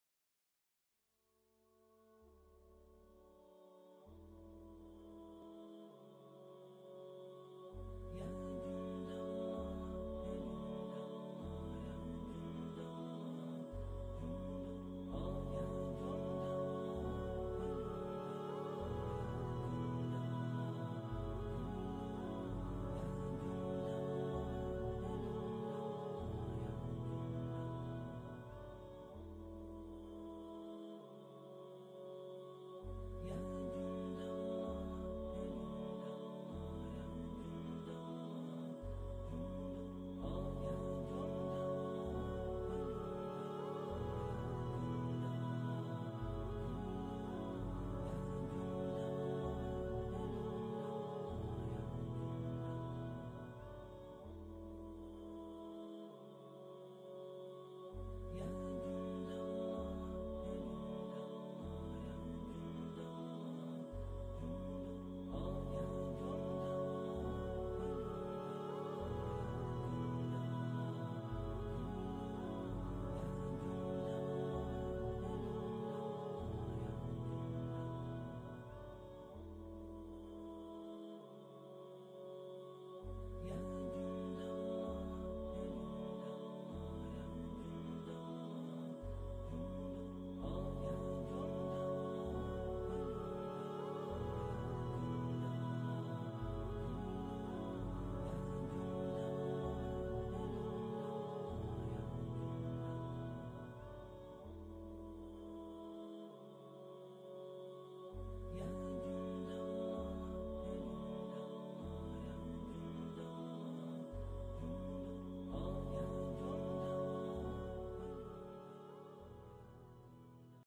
5 хадисов о силе молитвы
Как часто, будучи в сложных ситуациях, мы обращаемся к Богу, и проблема решается. Бывает и так, что после мольбы сердце успокаивается, и сложное становится легким.